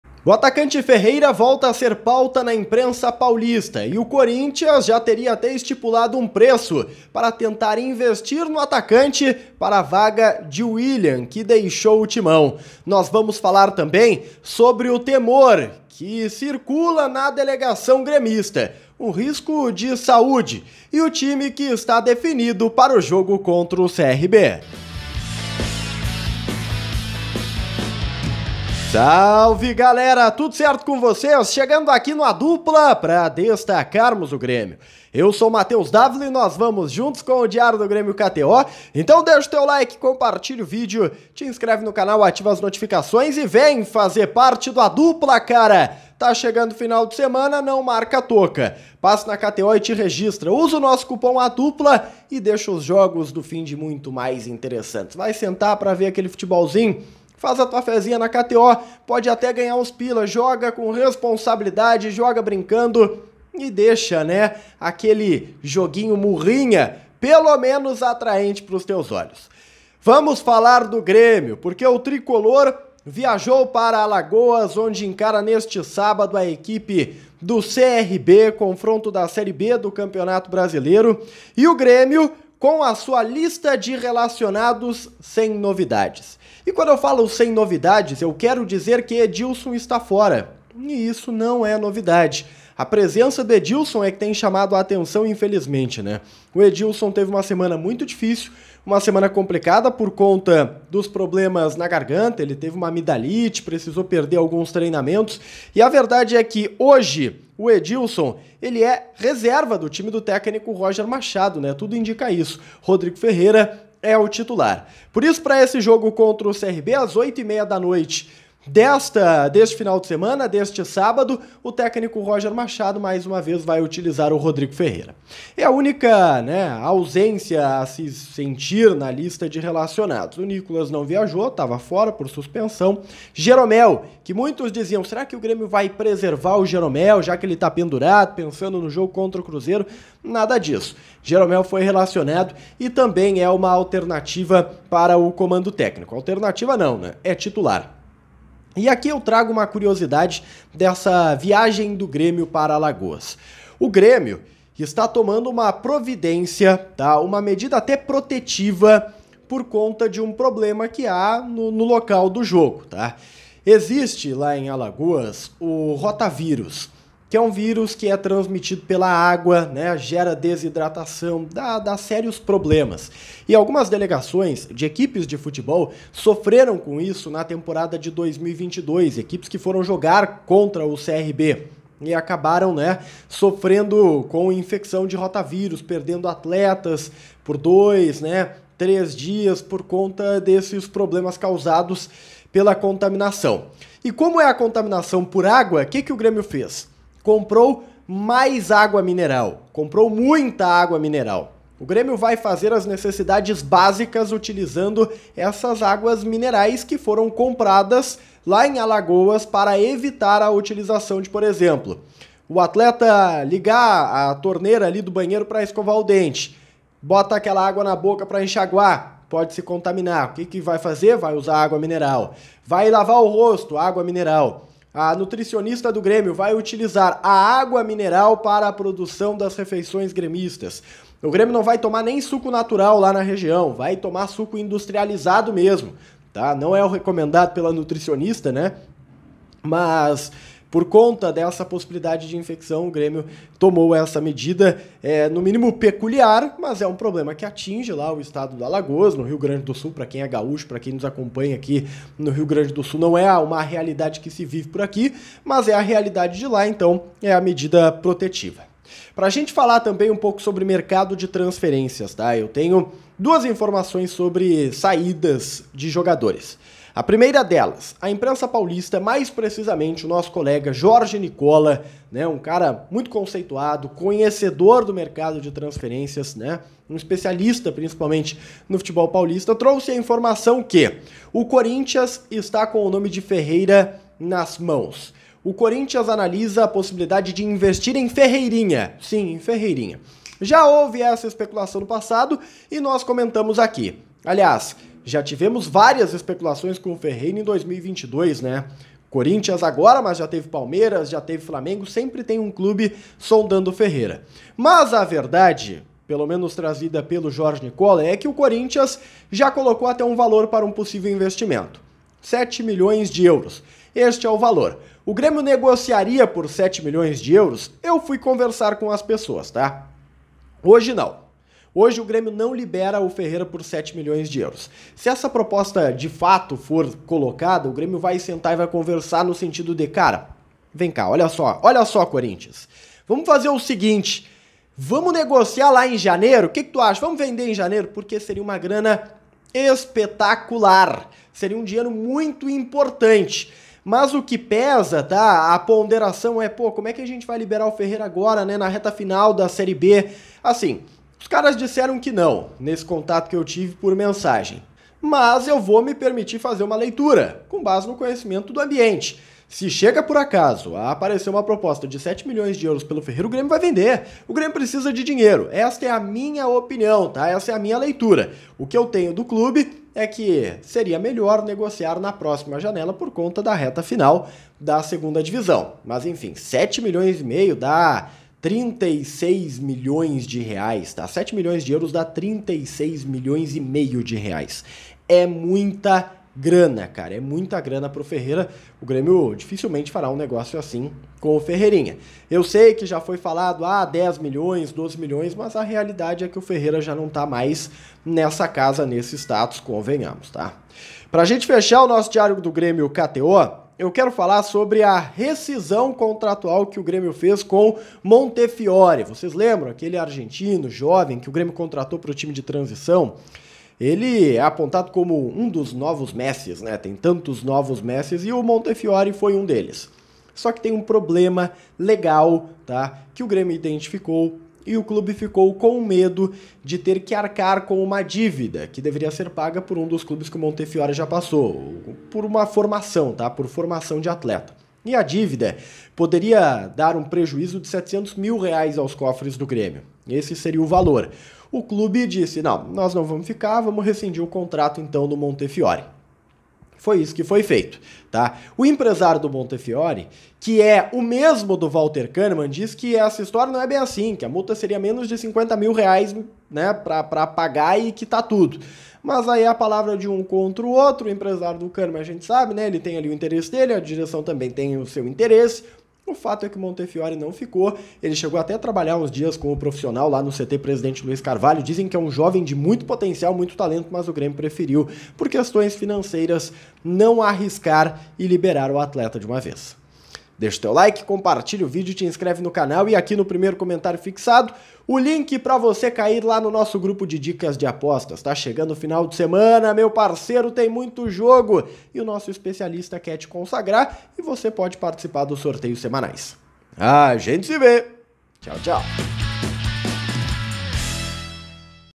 🔵⚫ Diário do Grêmio KTO: Corinthians vê em Ferreira substituto de Willian | Perigo em Alagoas | Time
Use o cupom A DUPLA lá na KTO e ganhe 20% em cima do seu primeiro depósito.

AGORA PARECE SER ALGO SÉRIO. E AÍ, LIBERA O FERREIRINHA PARA O TIMÃO?

Segue o D'Avila no instagram: @davilamatheus

Thumb: Lucas Uebel / Grêmio FBPA